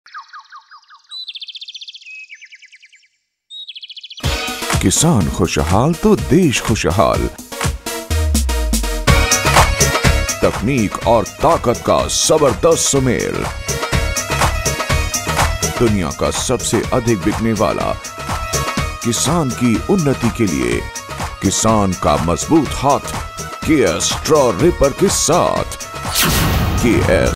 KS 756 DB Straw Reaper by KS AGROTECH Private Limited.
KS 756 DB Straw Reaper by KS AGROTECH Private Limited. Manufacturer of Combine Harvesters, Straw Reapers (Small Harvester Combine), Rotavators / Rotar Tillers, Roto Seed Drills and Laser Land Levelers.